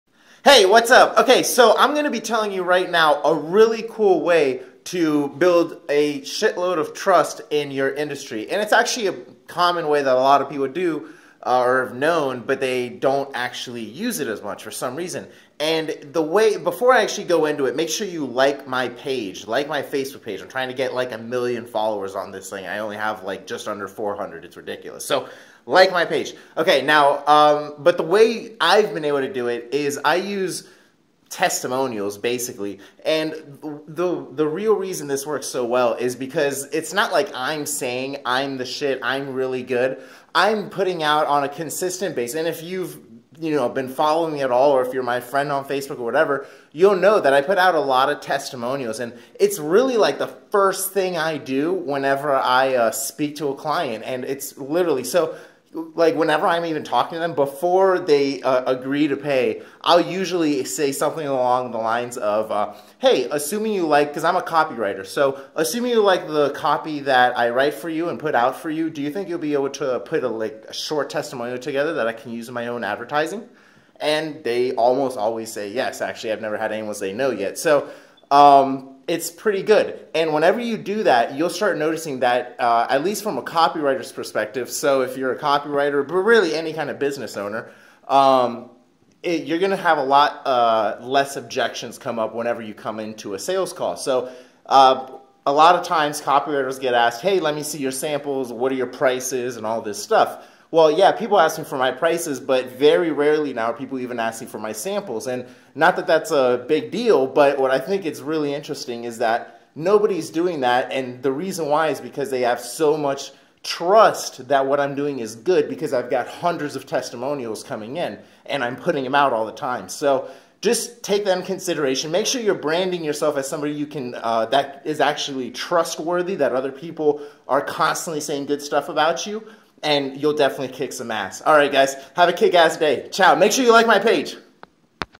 Copywriting - How To Build Trust
in this video I show you how to build trust and eliminate objections before they come up.